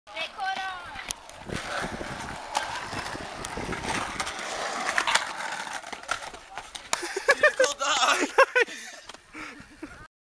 fukin fruit booters.MOV
roller blader gets in the way of what would have been a mctwist !!!